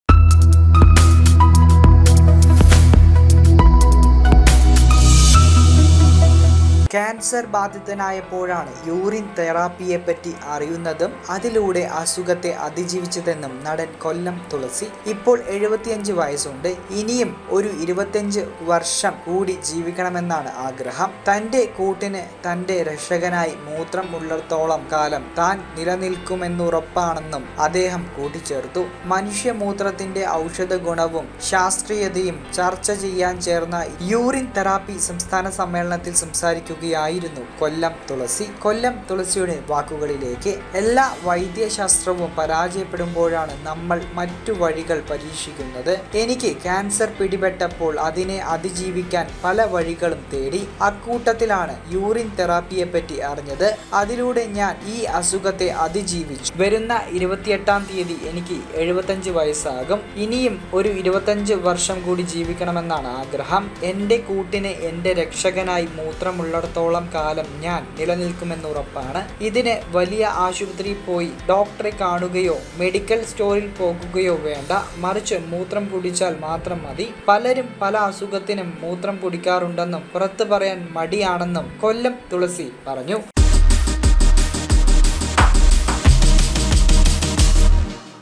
'മൂത്രം' കുടിച്ചാൽ മാത്രം മതി:കൊല്ലം തുളസി
കാൻസർ ബാധിതനായപ്പോഴാണ് യൂറിൻ തെറാപ്പിയെപ്പറ്റി അറിയുന്നതും അതിലൂടെ അസുഖത്തെ അതിജീവിച്ചതെന്നും നടൻ കൊല്ലം തുളസി. ഇപ്പോൾ 75 വയസ്സുണ്ട്, ഇനിയും ഒരു 25 വർഷം കൂടി ജീവിക്കണമെന്നാണ് ആഗ്രഹം. തന്റെ കൂട്ടിന് തന്റെ രക്ഷകനായി മൂത്രം ഉള്ളിടത്തോളം കാലം താൻ നിൽക്കുമെന്നുറപ്പാണെന്നും അദ്ദേഹം കൂട്ടിച്ചേർത്തു.മനുഷ്യ മൂത്രത്തിന്റെ ഔഷധഗുണവും ശാസ്ത്രീയതയും ചർച്ച ചെയ്യാൻ ചേർന്ന യൂറിൻ തെറാപ്പി സംസ്ഥാന സമ്മേളനത്തിൽ സംസാരിക്കുകയായിരുന്നു കൊല്ലം തുളസി.